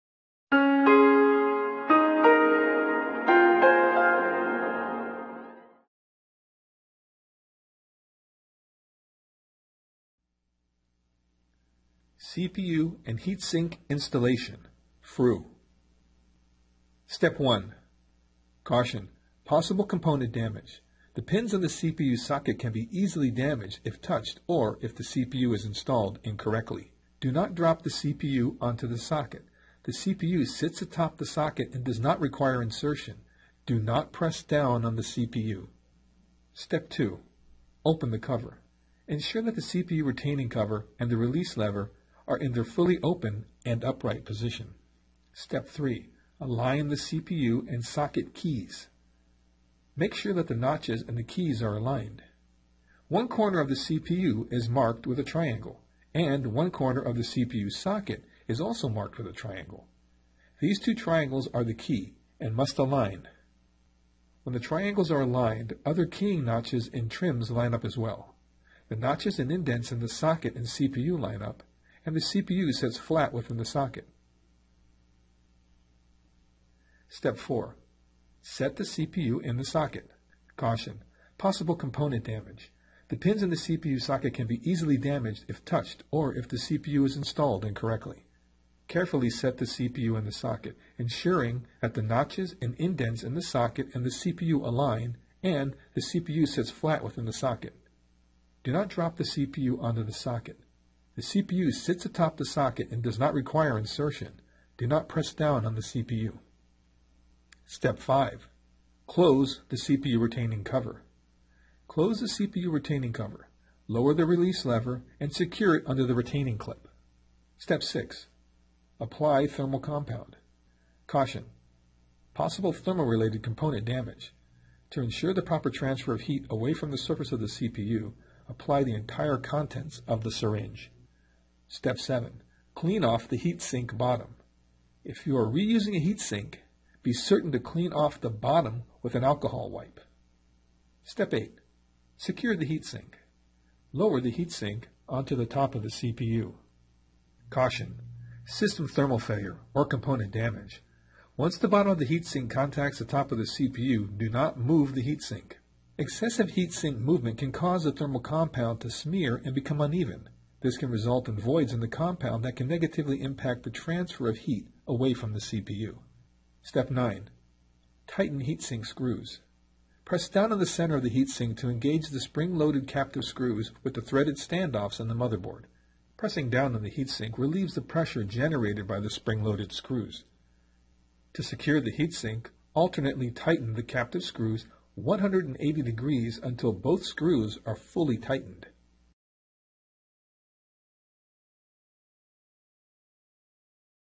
Sun Server X2-8: Installing a CPU and Heatsink (Field-Replaceable Unit)
Shows how to install a CPU and heatsink on Sun Server X2-8. This is a field-replaceable unit and should only be installed by Oracle field service.